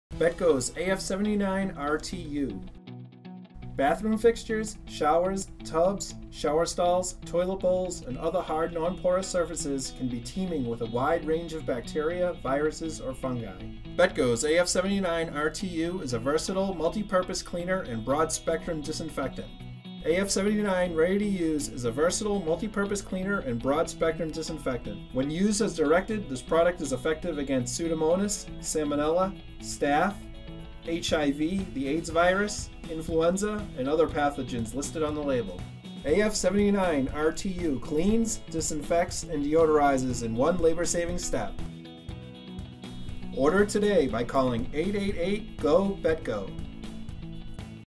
Betco AF79 R T U Disinfectant
Bectco's AF79 Ready-to-Use is an acid free bathroom cleaner, disinfectant and deodorant. It also Kills Pandemic 2009 H1N1 influenza A virus (formerly called swine flu).